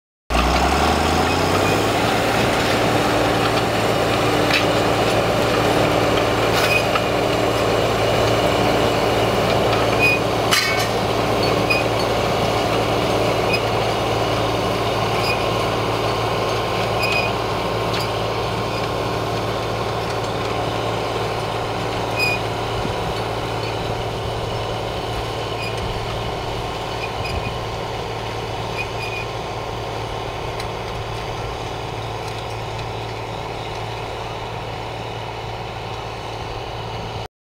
Offset disc seedbed preparation
This offset disc is preparing a seedbed in crested wheatgrass. It has two gangs of discs set at opposing angles to allow mixing of soil and residue. One set of discs throws the soil outward and the other throws the soil inward. It is tilling the soil to a depth of four to six inches.